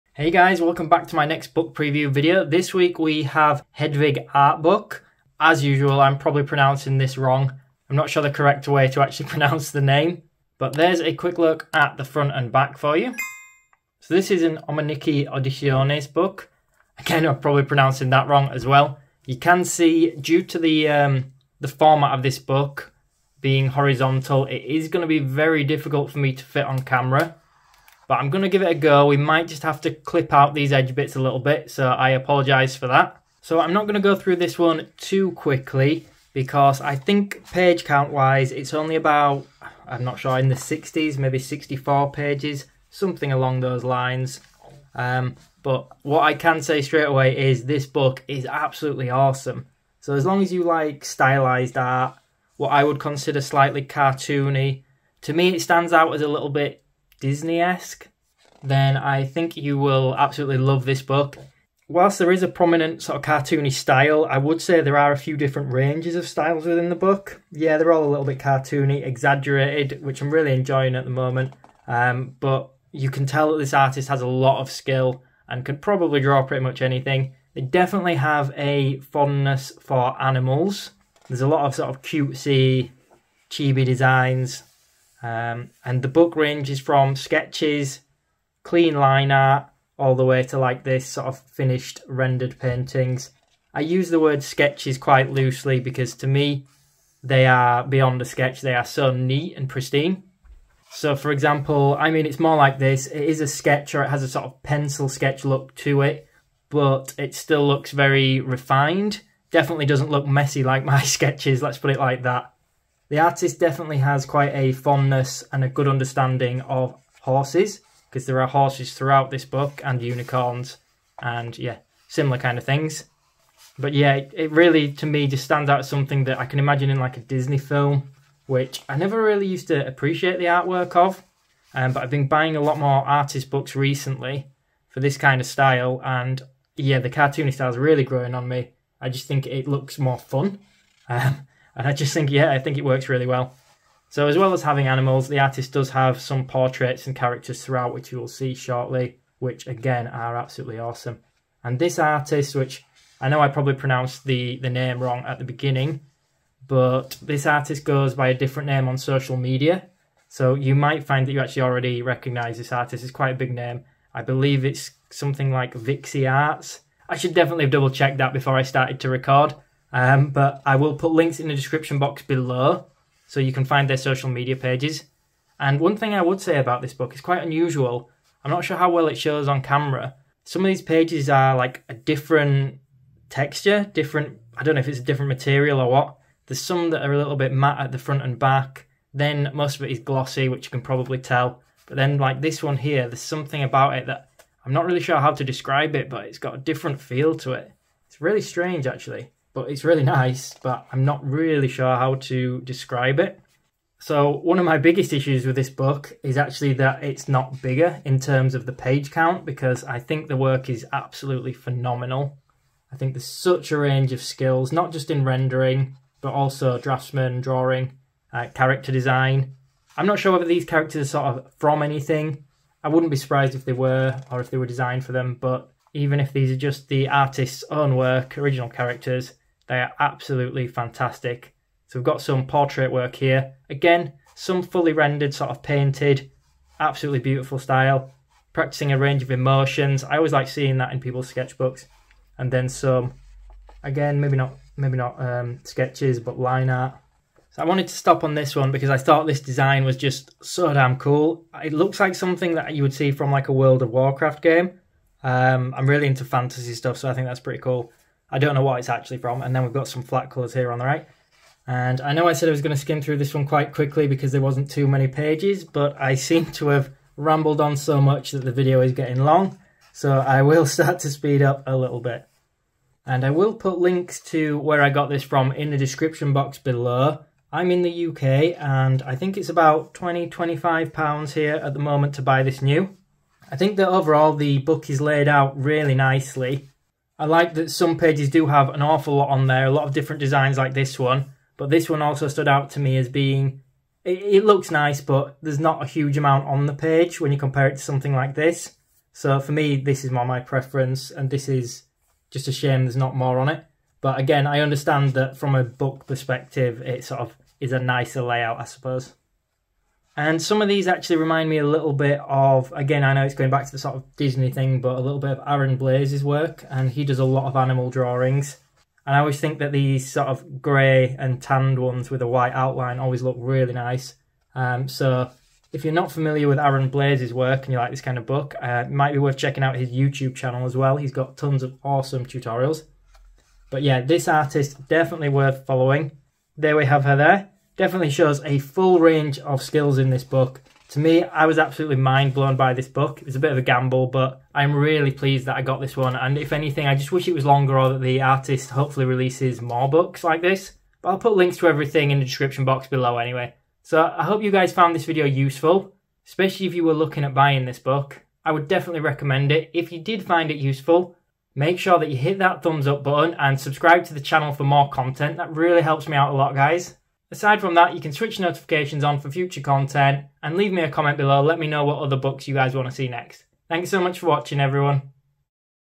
Hedvig Art Book VIXIEARTs art book preview amazing Disney style art work
Hedvig art book preview AKA VIXIEARTS sketchbook flick through - What an incredible Disney esk style, from sketches to fully rendered and painted pieces & characters. This artist shows incredible talent and drawing experience especially when it comes to cartoony characters and animal drawings (including chibi / kawaii animals).  This book will blow you away if you like exaggerated cartoony styles like the classic Disney princess and animals look. It's a shame that this book isn't bigger , longer because I really love all the drawings and painting. What did you guys think?